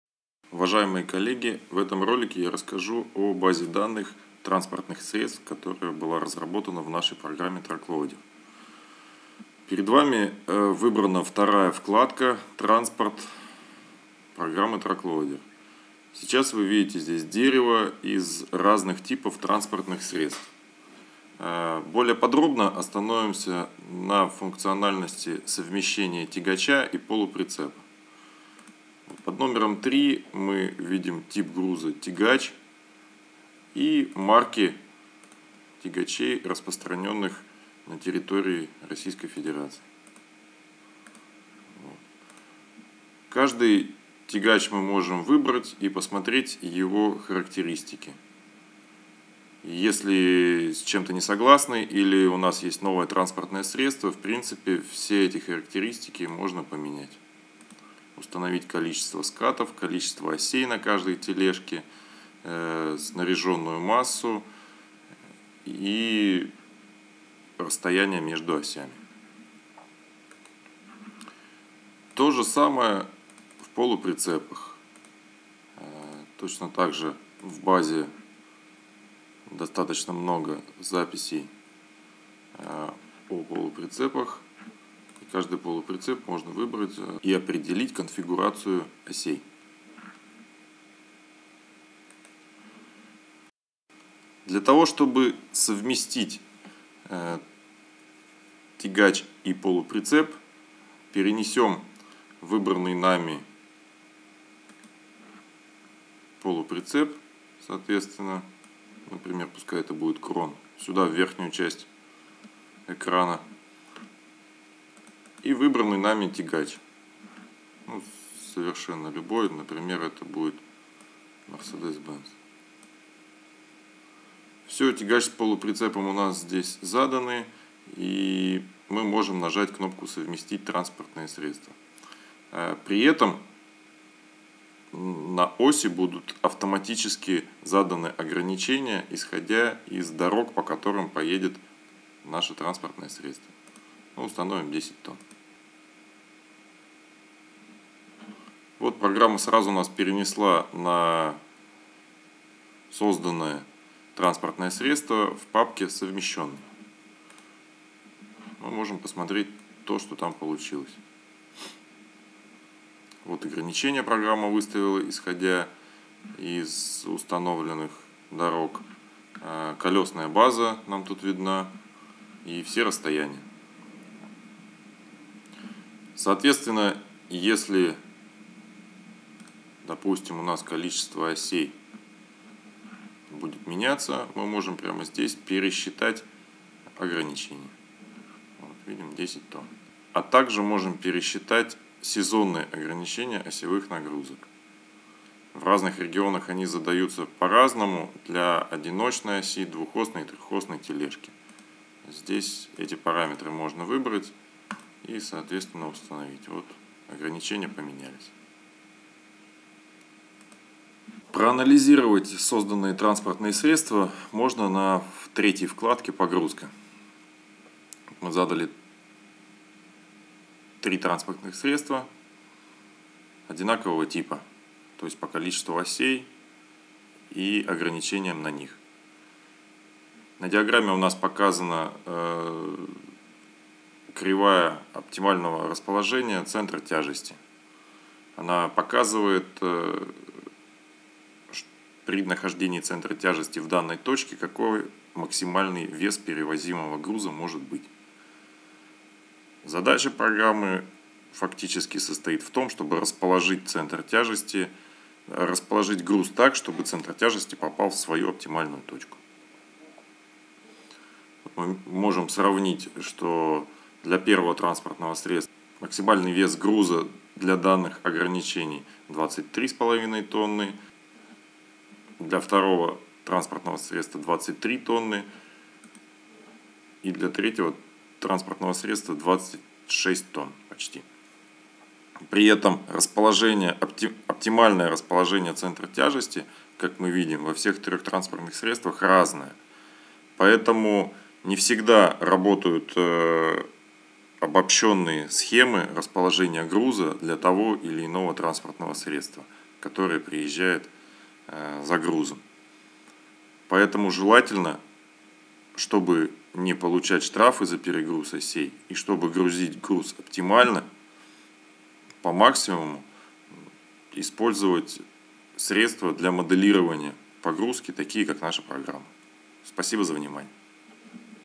Тягач + Полуприцеп + Ограничения на оси. База данных транспортных средств
Более 500 записей в новой базе транспортных средств TruckLoader и она постоянно пополняется. Можно составить автопоезд из тягача и полуприцепа.

Программа автоматически проставит ограничения на оси и оптимизирует расположение груза, чтобы максимально загрузить и не получить штраф.

Рассчитывайте нагрузки на оси до выполнения погрузки! Это сэкономит вам время и убережет от штрафов за перегруз.